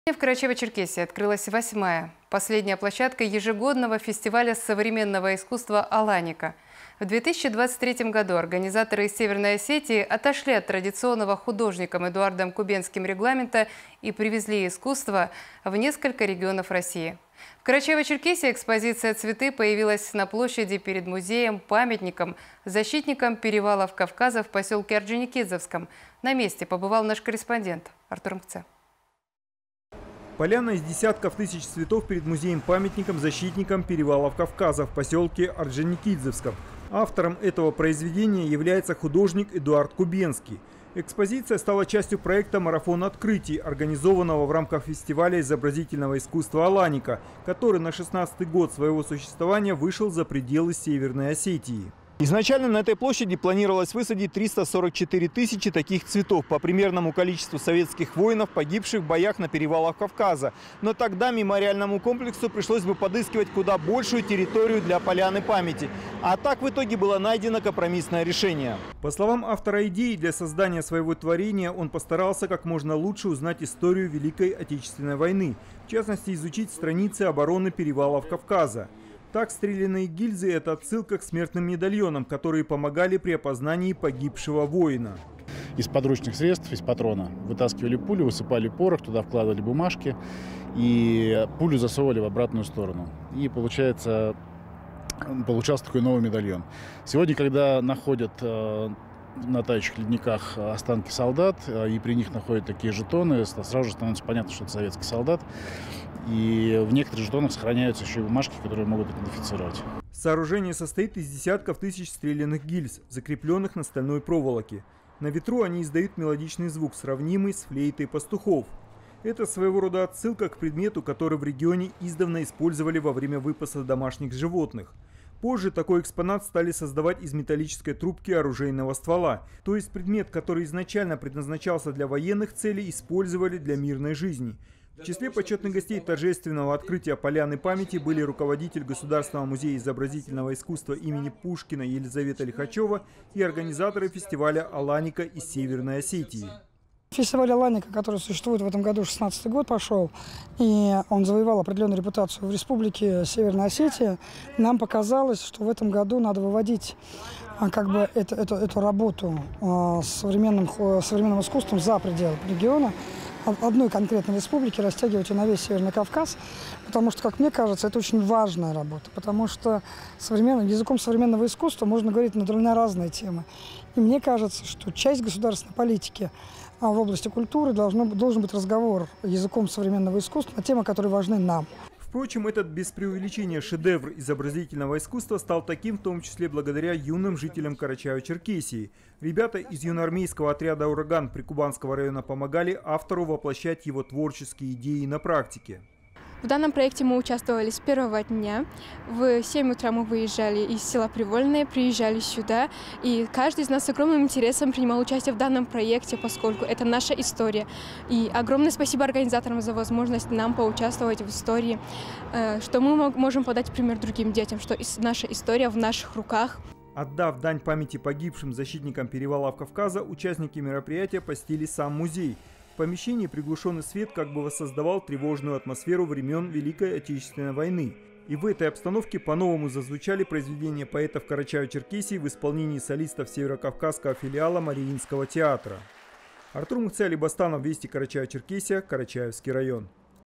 Поляна памяти "Аланики" в Карачаево-Черкесии
В музее-памятнике защитникам перевалов Кавказа в Карачаево-Черкесии открылась последняя, восьмая, плошадка ежегодного фестиваля современного искусства "Аланика". В 2023-м организаторы форума отошли от традиционного регламента и "привезли" искусство в другие регионы России. В нашей республике перед площадью мемориального сооружения была "высажена" поляна цветов. Автором проекта является художник из Екатеринбурга Евгений Кубенский. Установка композиции, а это 30 тысяч цветов из стреляных гильз, была проведена при участии воспитанников Юнармии республики